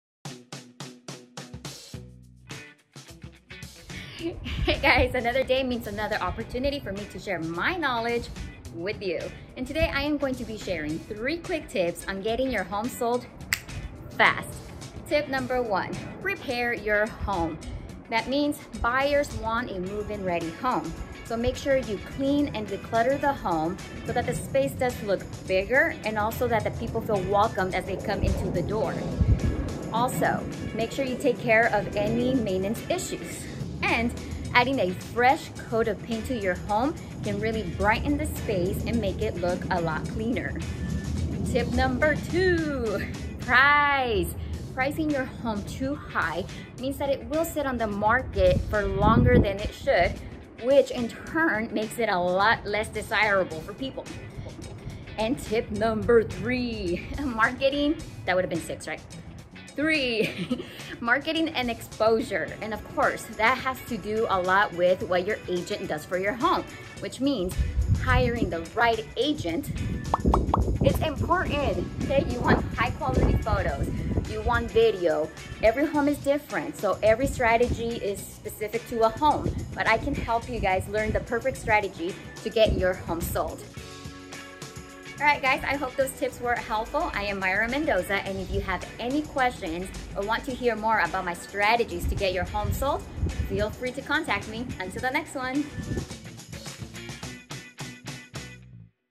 How to Get Your Home Sold Fast - Selling San Diego
Selling a house can be stressful. Finding the right real estate agent to partner with to market your home is the first step and one that should not be taken lightly. But, the selling process shouldn’t be placed entirely in your agent’s lap.

There are several things that every homeowner can (and should) do to make their property standout since nobody wants a house for sale that sits on the market. Here are three tips to make sure your property stands out from its competition even in an over-saturated or off-season market that will ensure that you sell your home as quickly as possible for the highest possible price.

1. Prepare Your Home 🏡
2. Price It To Sell 💰
3. Marketing & Exposure 📸